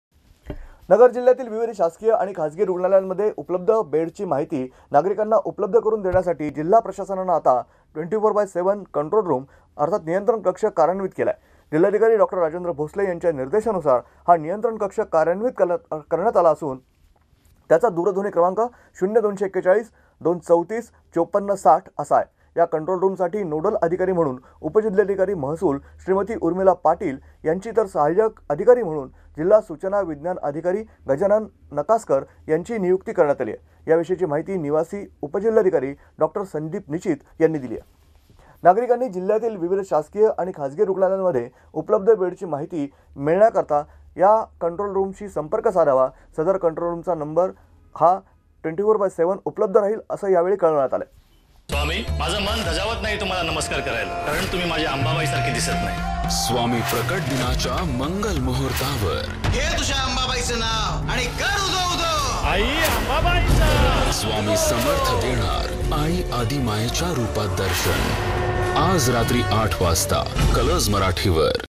कोरोना कंट्रोल रूम कार्यान्वित ,24x7 अक्टीव्ह कोरोना कंट्रोल रूम
जिल्हयातील विविध शासकीय व खाजगी रुग्णालयांमध्ये उपलब्ध बेइची माहिती नागरिकांना उपलब्ध करुन देण्यासाठी जिल्हा प्रशासनाने आता  24 x 7 कंट्रोल रूम (नियंत्रण कक्ष) कार्यान्वित करण्यात आला आहे. जिल्हाधिकारी डॉ. राजेन्द्र भोसले यांच्या निर्देशानुसार हा नियंत्रण कक्ष कार्यान्वित करण्यात आला असून  त्याचा दूरध्वनी क्रमांक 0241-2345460  असा आहे. या कंट्रोल रुमसाठी नोडल अधिकारी म्हणून उपजिल्हाधिकारी (महसूल) श्रीमती उर्मिला पाटील यांची तर सहाय्यक अधिकारी म्हणून जिल्हा सूचना विज्ञान अधिकारी गजानन नकासकर यांची नियुक्ती करण्यात आली आहे, अशी माहिती निवासी उपजिल्हाधिकारी संदिप निचित यांनी दिली आहे.
नागरीकांनी जिल्हयातील विविध शासकीय व खाजगी रुग्णालयांमध्ये उपलब्ध बेडची माहिती
मिळणेकरीता सदर कंट्रोल रुमशी संपर्क साधावा. सदर कंट्रोल रुम 24 X7 सुरु राहील, असे कळविण्यात आले आहे